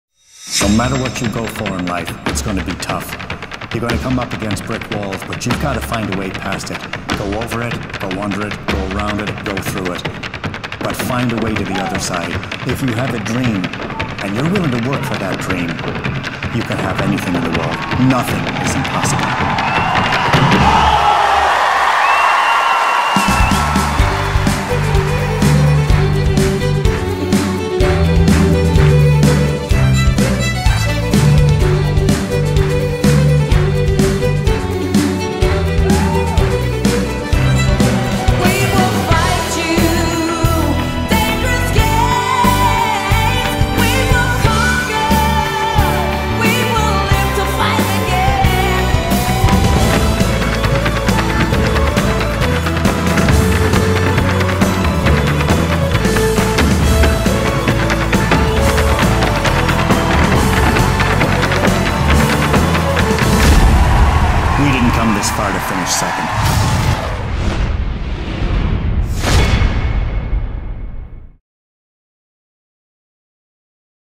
Lord Of The Dance - Dangerous Games Trailer
Michael Flatley's Lord of the Dance - Dangerous Games opening at the Dominion Theatre in March 2015.